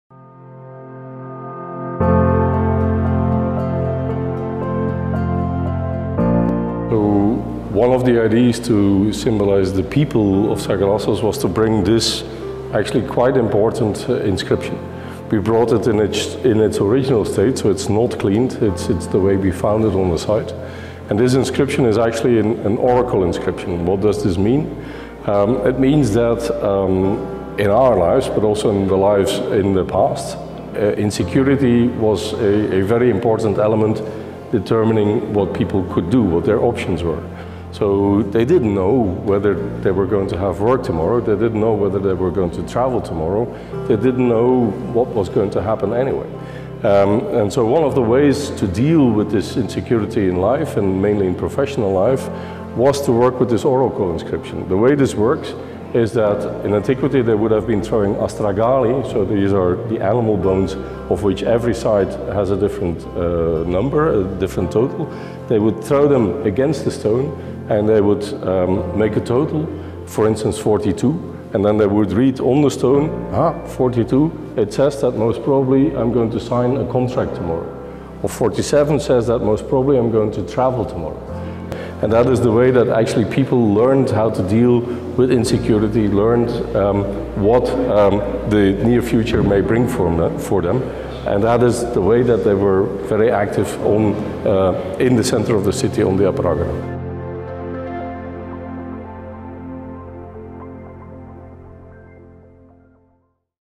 Meanwhile in the Mountains: Sagalassos - The Astragalos Oracle | Turkish Museums
If you wonder what the near future brings for you, you may visit the exhibition to see this oracle inscription in İstanbul.